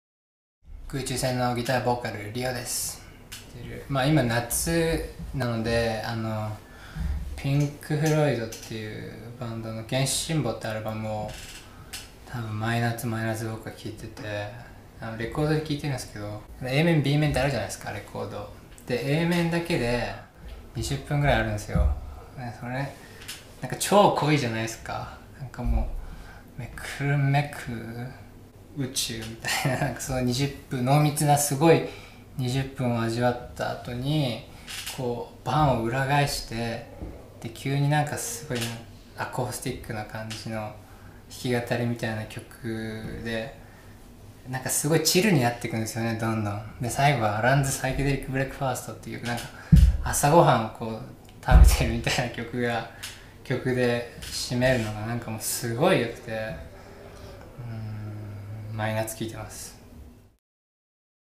今週のヘビロテVol.24 クウチュウ戦、リヨ
クウチュウ戦、リヨが最近よく聴いている、ハマっている曲、もしくはずっと昔から好きな曲を紹介!

ロックを軸にしたWEBマガジン[Rock is -ロックイズ]